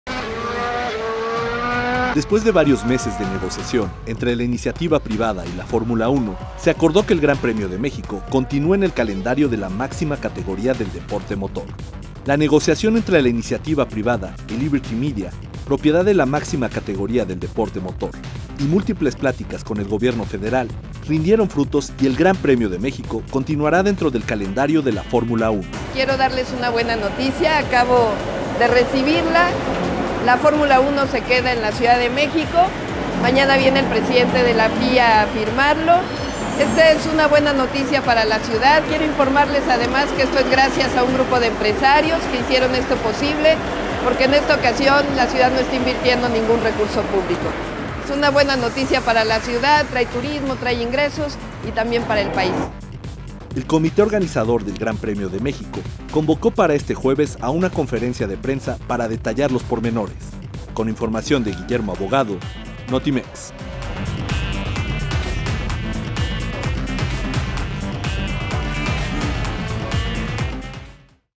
Fórmula 1 se queda en la CDMX
El comité organizador del Gran Premio de México convocó para este jueves a una conferencia de prensa para detallar los pormenores.